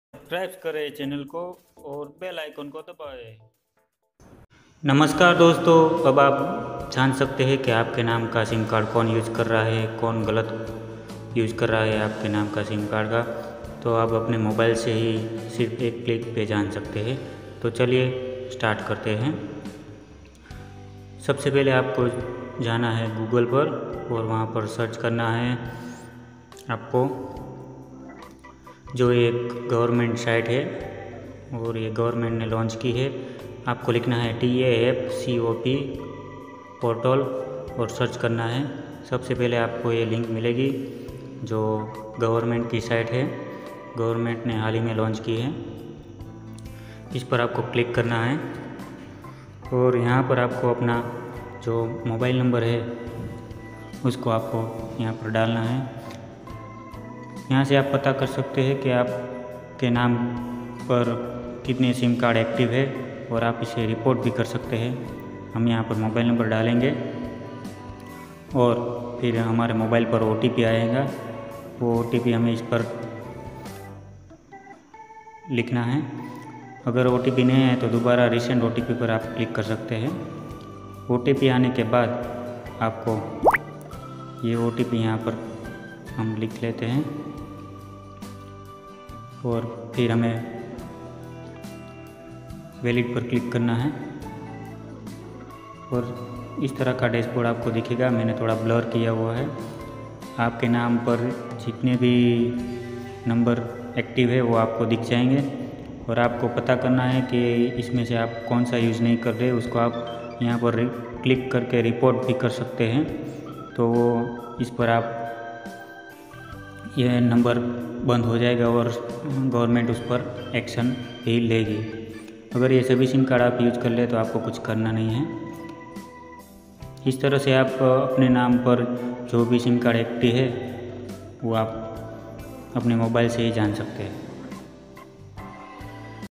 आपके नाम का सिम कार्ड कौन यूज कर रहा है जानिए एक ही क्लिक पर।। 2021 how to know your active sim
दोस्तों अब आप अपने मोबाइल से ही जान सकते हैं कि आपके नाम पर कितनी सिम कार्ड एक्टिव है और आप के नाम पर किसने सिम कार्ड ले लिया है तो आप जान सकते हैं कि आपका नाम का सिम कार्ड कोई गलत यूज कर रहा है तो आप इसे रिपोर्ट भी कर सकते हैं और इसके लिए आपको एक गवर्नमेंट की साइट है जो गवर्नमेंट ने हाल ही में लॉन्च की है उस पर जाना होगा और वहां पर आपको अपने मोबाइल नंबर डालकर आप अपने मोबाइल नंबर से आपके नाम पर कितनी सिम कार्ड एक्टिव है और आप सिम यूज नही कर रहे हो तो एप रिपोर्ट भी कर सकते हो

दोस्तों मैंने जो जो लिंक दिए इस पर आप क्लिक करके आपके नाम पर एक्टिवेशन करें उसका पता कर सकते हैं इस लिंक पर क्लिक कीजिए और मोबाइल नंबर और ओटीपी डालिए
how to check any sim data